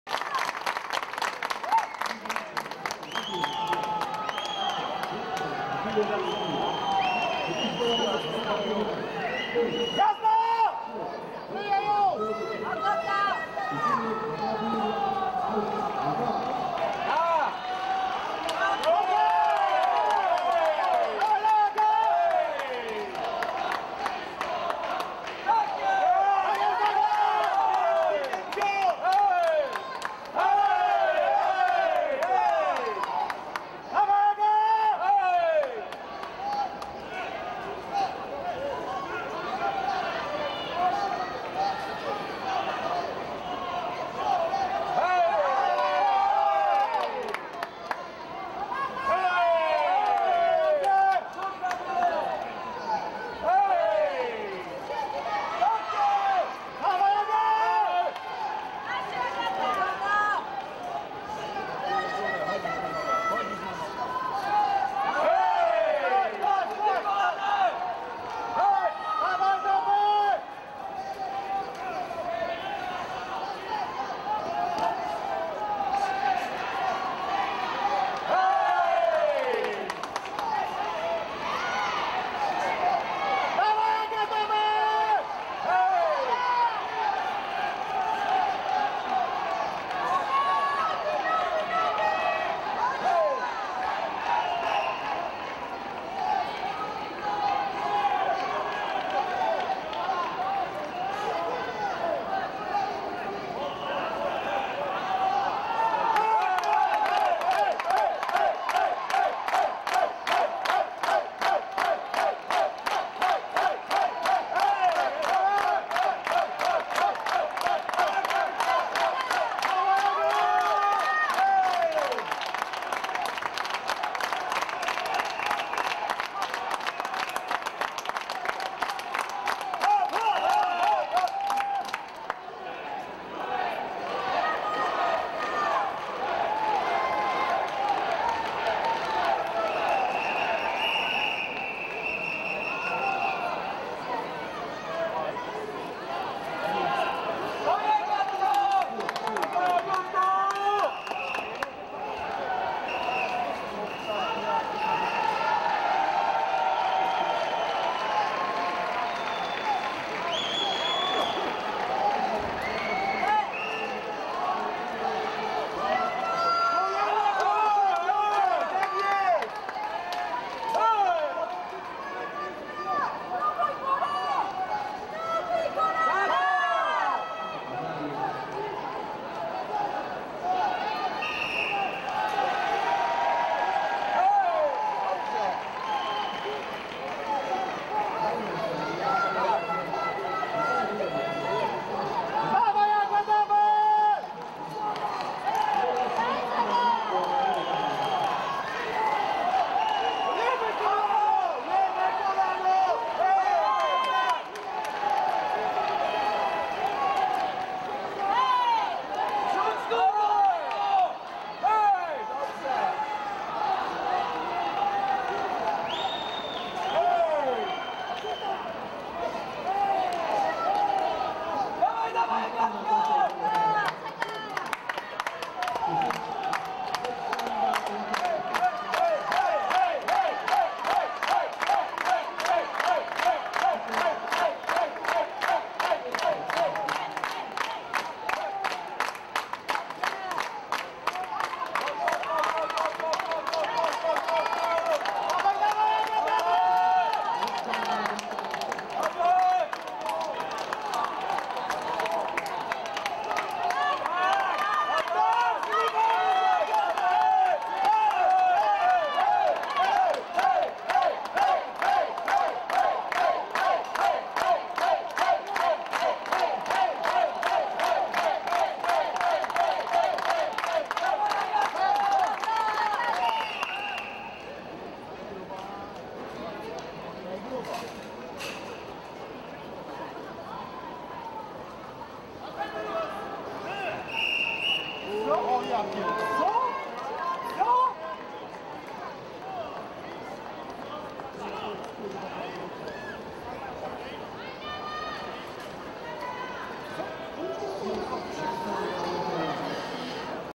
Court1 2nd Round7 woman Agata Winiarska vs Azhar Zhaxybayeva The 12th World Karate Championship
2019年11月9日（土）、10日（日）
第12回全世界空手道選手権大会
第1コート　女子2回戦
アガタ・ヴィニアルスカ (ポーランド) vs. アジャール・ジャクシバイェワ (カザフスタン)

The 12th World Karate Championship
Agata  Winiarska (Poland) vs. Azhar Zhaxybayeva  (Kazakhstan )

第12回世界大会プレイリスト／12th World Championship playlist